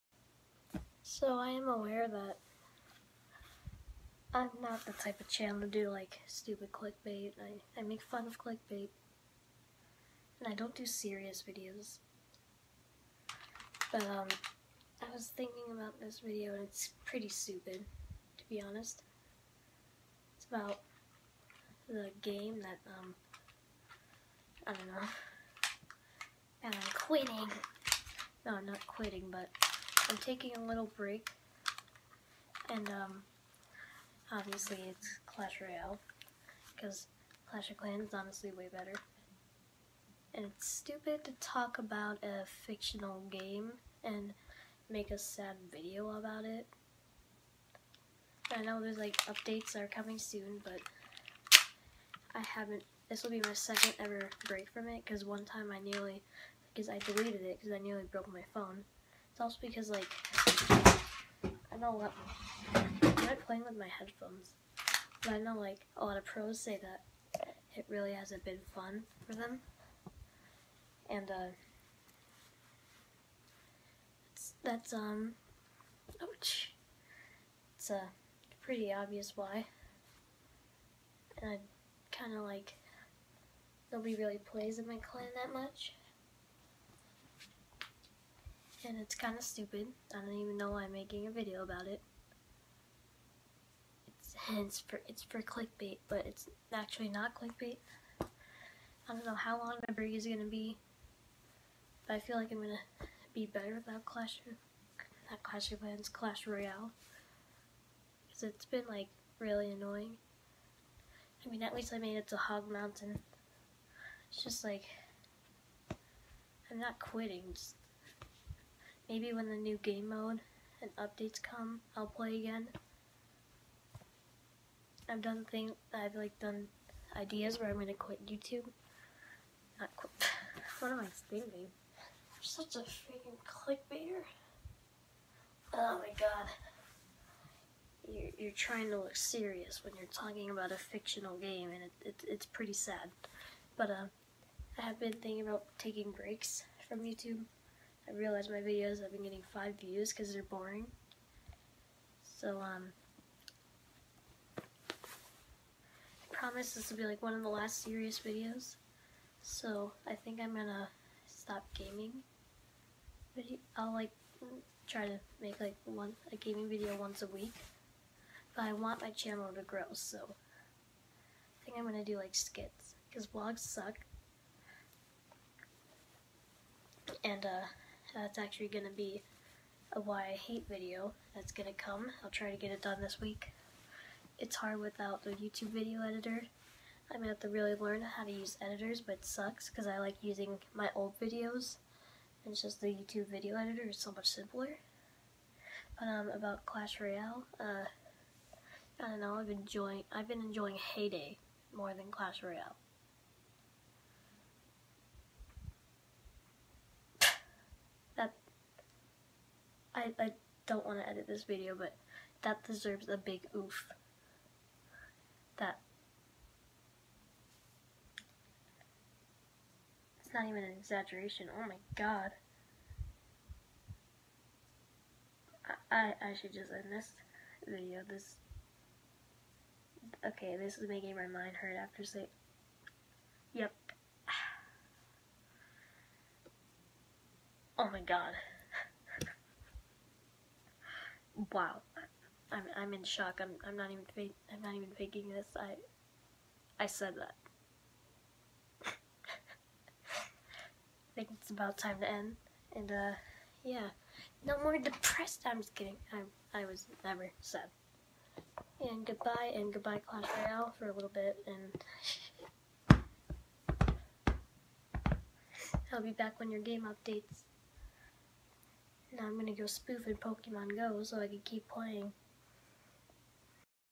I’m quitting Clash Royale for a while + channel change!
Just your 12 year who thinks he’s better than everyone else, talking about important things while having an empty Lunchables container next to him.